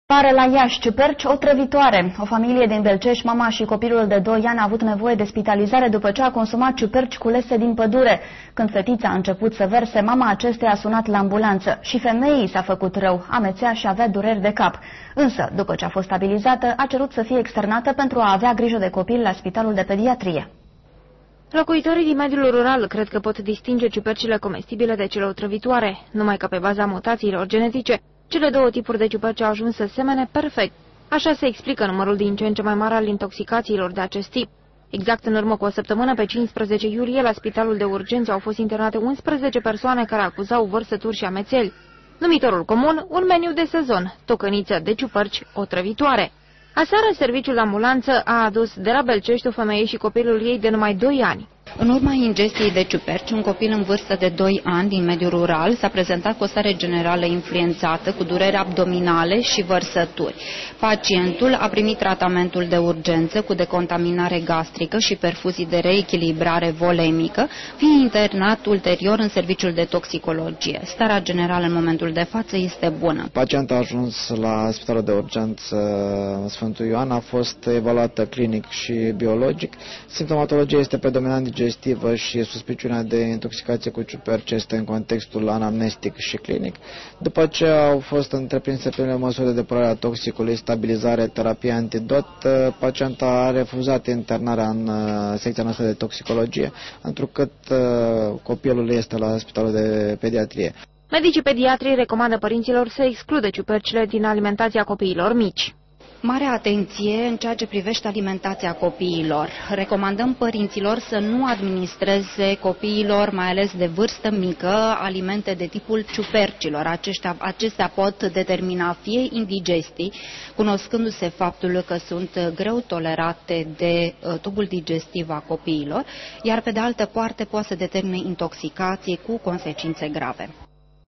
Meniul de vara la Iasi - ciuperci otravitoare
Dr. Tudor Ciuhodaru, coordonator UPU Spitalul "Sfantul Ioan": "Pacienta a ajuns la Spitalul de Urgente "Sfantul Ioan" , a fost evaluata clinic si biologic , simptomatologia este predominant digestiva , suspiciunea de intoxicatie cu ciuperci este in contextul anamnestic .Dupa ce au fost efectuate primele masuri de depurare a toxicului , stabilizare, terapie antidot pacienta a refuzat internarea in  Sectia noastra de Toxicologie  intrucat copilul este Spitalul de Pediatrie."